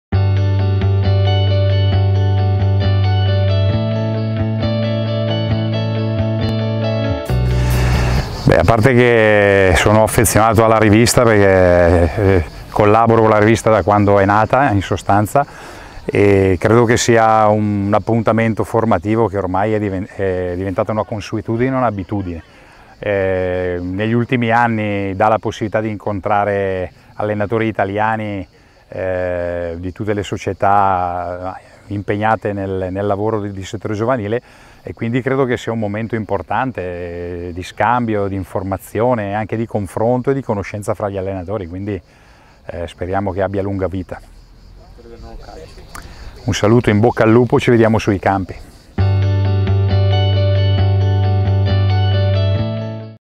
Master Settore Giovanile 2018: parlano i relatori
Stefano Bonaccorso, responsabile dell'attività di base dell'Atalanta